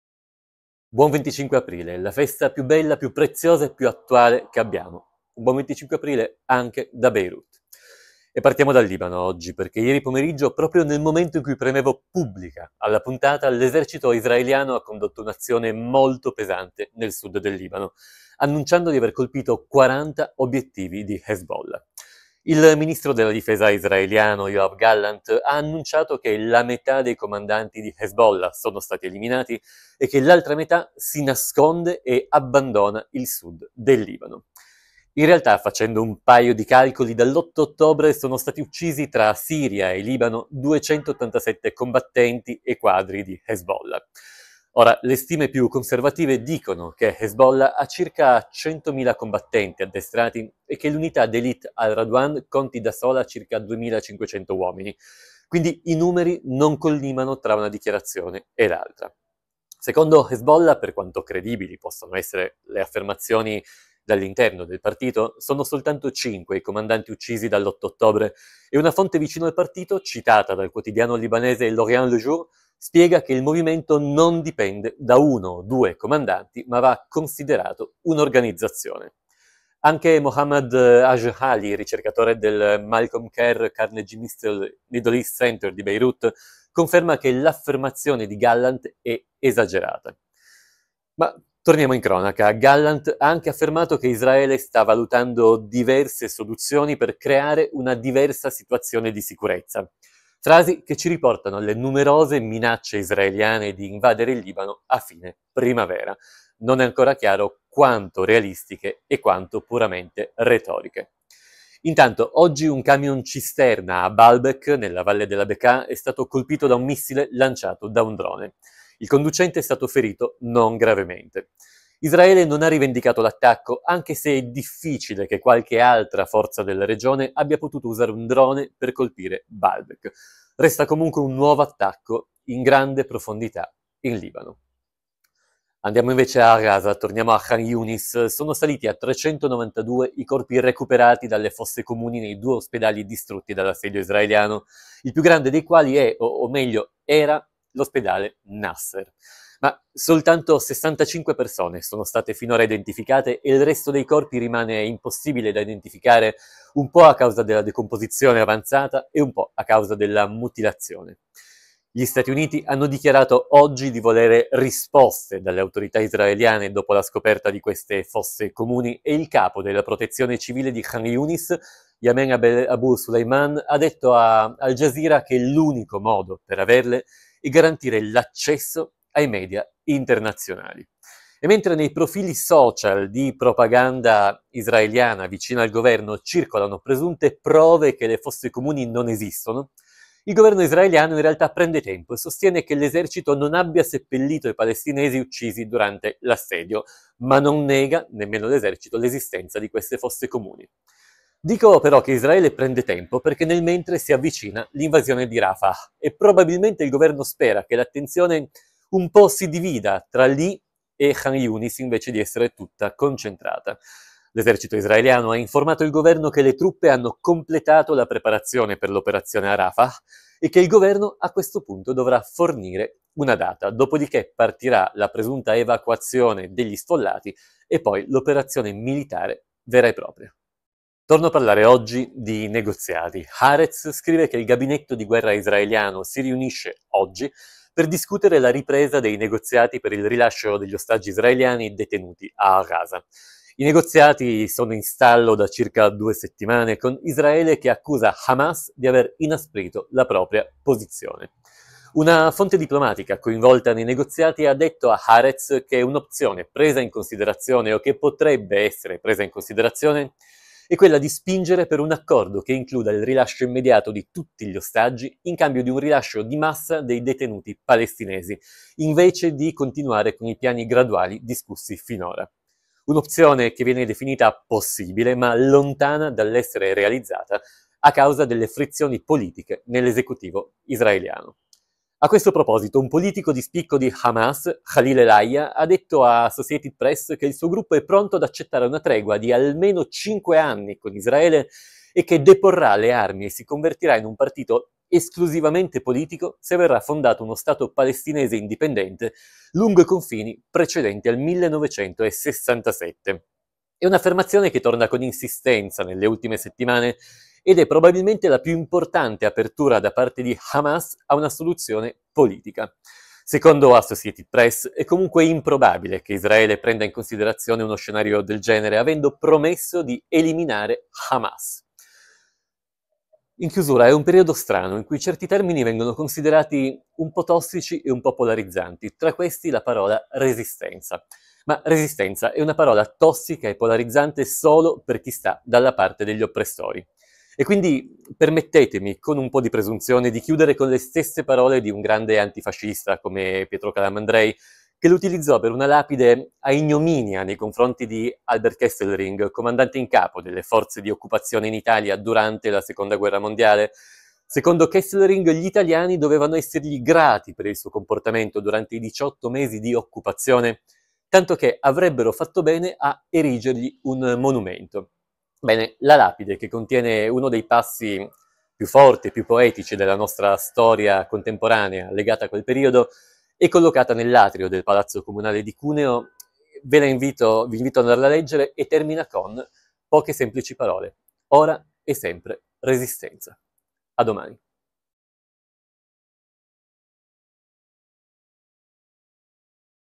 #158 - Mosse a sorpresa
Buon 25 aprile, la festa più bella, preziosa e attuale che abbiamo, anche da Beirut.

Ieri pomeriggio, proprio nel momento in cui premevo “pubblica” della puntata, l’esercito israeliano ha condotto un’azione molto pesante nel sud del Libano, annunciando di aver colpito 40 obiettivi di Hezbollah.

Gallant ha anche affermato che Israele sta valutando “diverse soluzioni” per “creare una diversa situazione di sicurezza”, frasi che ci riportano alle numerose minacce israeliane di invadere il Libano a fine primavera.

A Khan Younis. Sono saliti a 392 i corpi recuperati dalle fosse comuni nei due ospedali distrutti dall’assedio israeliano, il più grande dei quali è, o meglio era, il Nasser.

Ma soltanto 65 persone sono state identificate finora, e il resto dei corpi rimane non identificato a causa della decomposizione avanzata o della mutilazione.

Gli USA hanno dichiarato oggi di volere “risposte”.

Ma intanto Israele prepara l’invasione di Rafah, e secondo l’esercito israeliano è tutto pronto, in attesa di una decisione politica.

Oggi il gabinetto di guerra israeliano si riunisce oggi per discutere la ripresa dei negoziati per il rilascio degli ostaggi israeliani detenuti a Gaza.

I negoziati sono in stallo da due settimane, con Israele che accusa Hamas di aver inasprito la propria posizione.

A questo proposito, Hamas ha detto ad Associated Press che il suo gruppo è pronto ad accettare una tregua di almeno 5 anni con Israele e che deporrà le armi e si convertirà in un partito esclusivamente politico se verrà fondato uno Stato palestinese indipendente lungo i confini pre-1967.